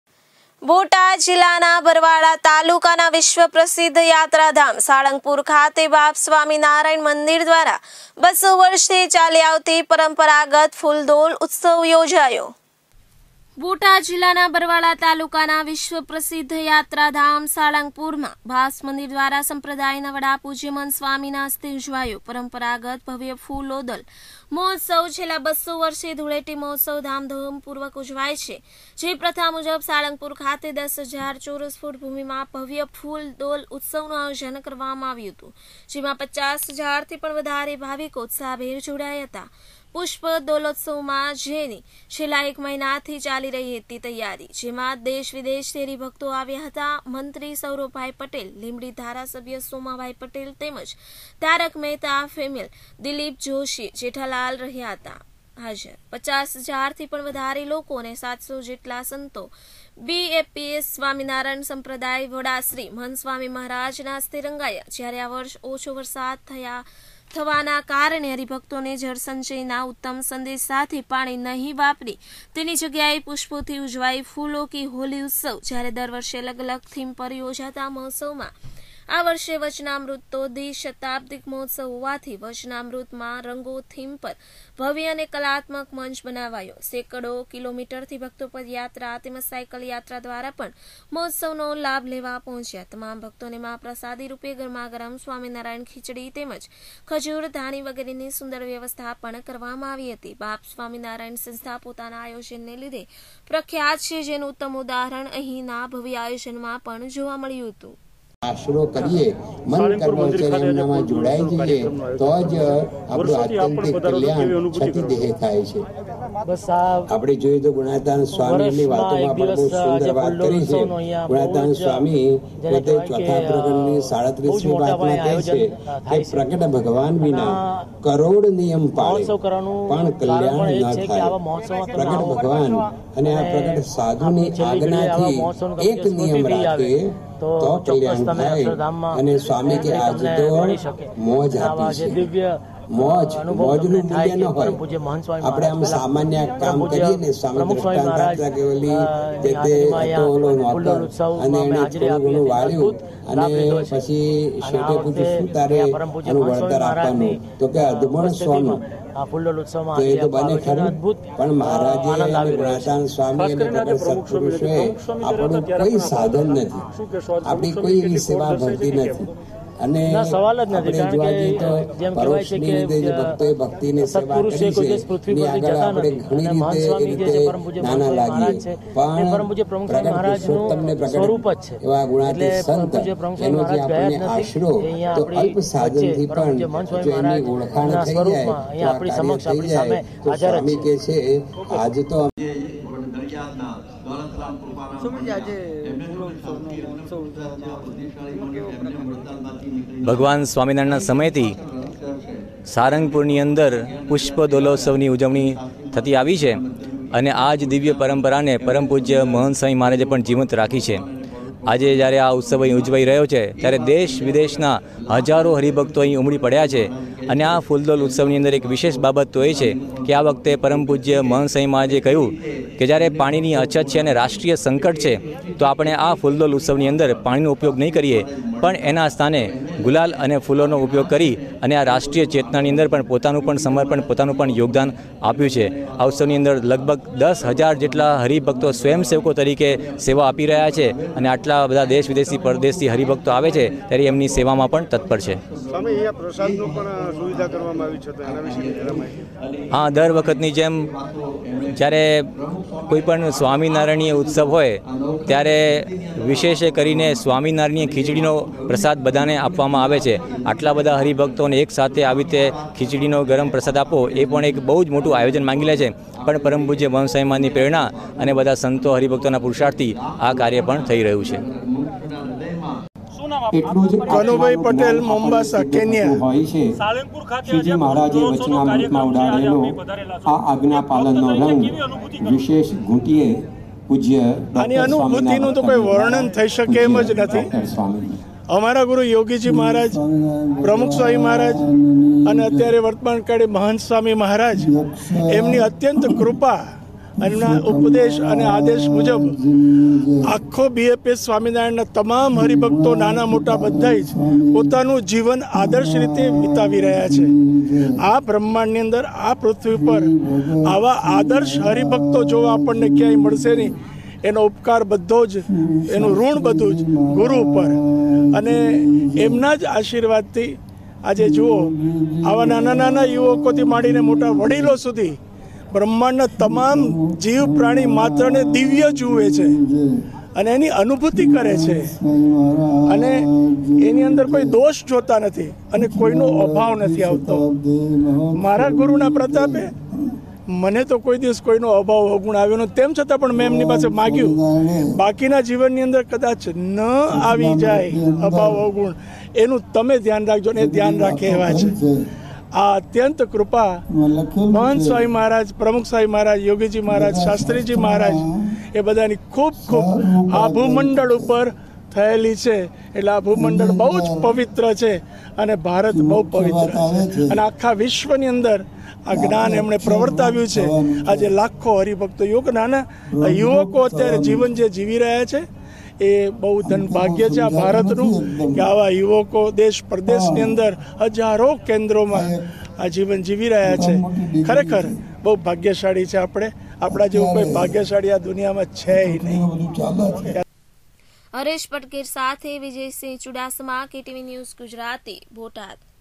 બોટાદ જિલ્લાનાં બરવાળા તાલુકાનાં વિશ્વપ્રસિધ્ધ યાત્રાધામ સાળંગપૂર ખાતે BAPS
બોટાદ જિલ્લાનાં બરવાળા તાલુકાનાં વિશ્વપ્રસિધ્ધ યાત્રાધામ સાળંગપૂર ખાતે BAPS સ્વામિનારાયણ મંદિર દ્વારા 200 વર્ષથી ચાલી આવતી પરંપરાગત ફૂલદોલ ઉત્સવ યોજ્યો